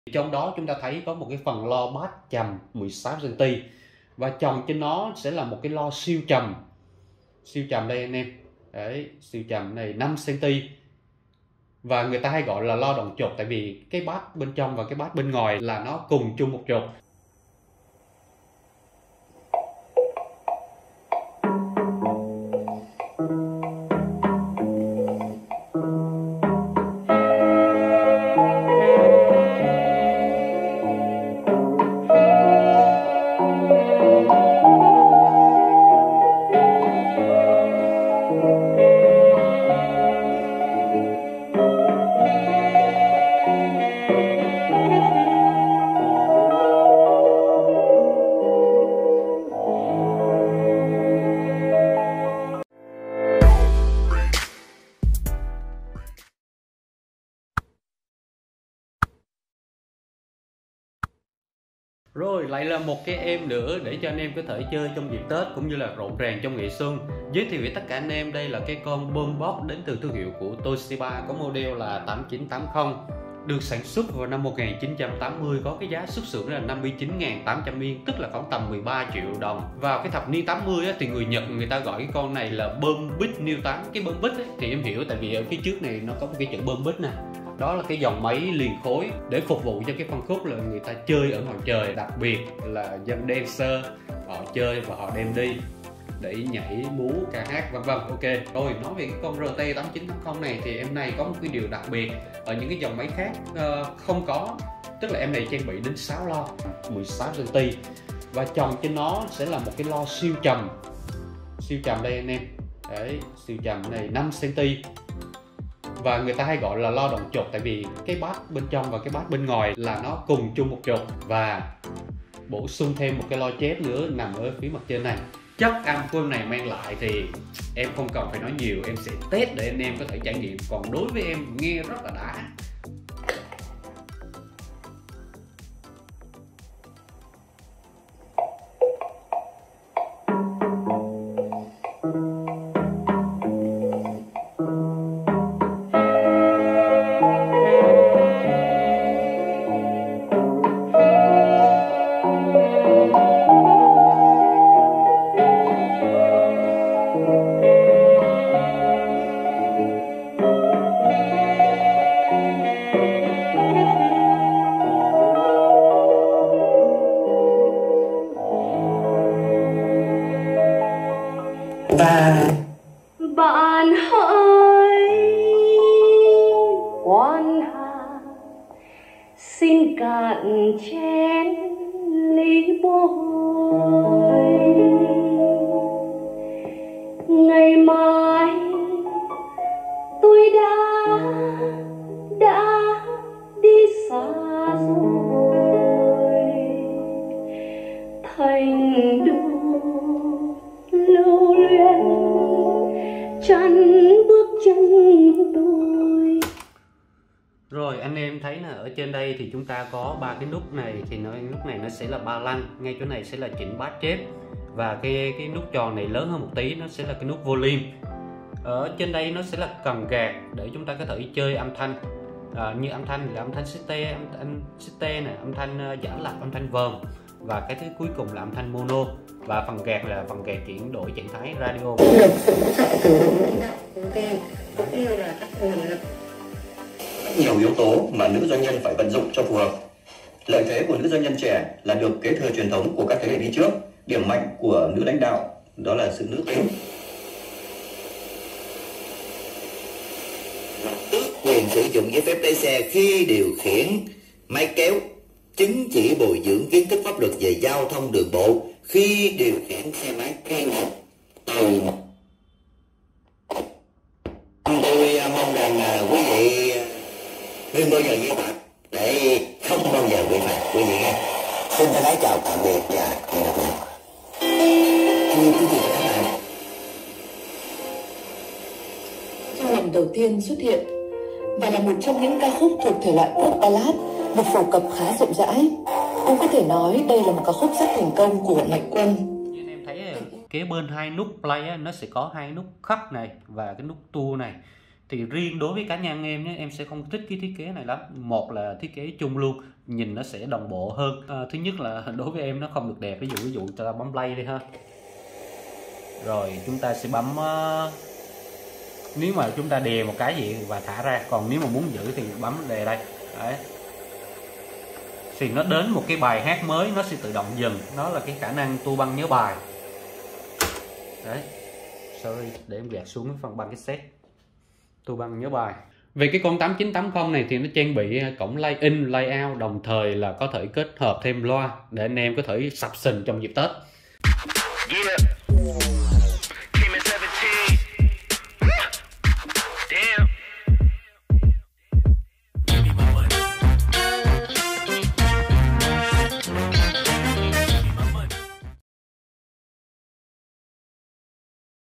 CASSETTE TOSHIBA RT-8980SM 6 Loa ( 2 ĐỒNG TRỤC, 2 treble) chất âm "KINH ĐIỂN" giá 4t5 || LP AUDIO
BOMBEAT CASSETTE TOSHIBA RT-8980SM với hệ thống 6 Loa trong đó có 1 cặp ĐỒNG TRỤC cho chất âm "TỚI CÔNG CHUYỆN"
CASSETTE TOSHIBA RT-8980SM sản xuất năm 1980 với giá xuất xưởng 59,800 yên
giao lưu 4t5
Xem sản phẩm:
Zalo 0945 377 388
- Radio đả kéo full tần số
- Chơi cassette cực hay, âm thanh béng
- Loa trầm 16cm, loa siêu trầm 5cm, loa Trelbe 4cm
- Kích thước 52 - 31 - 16 cm